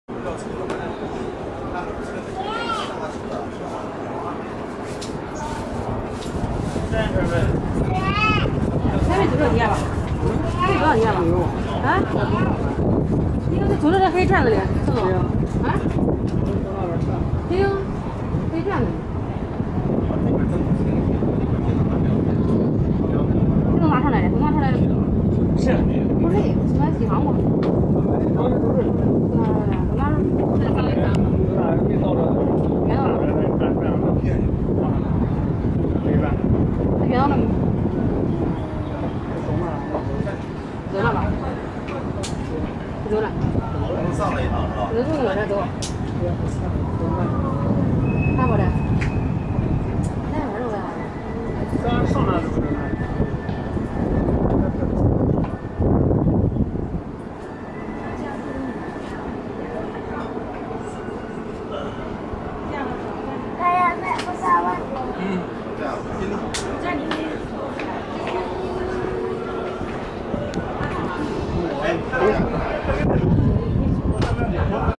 Brief look at West Lake in Hangzhou
Panning across the lake from atop the Leifeng Pagoda at the south end of the lake.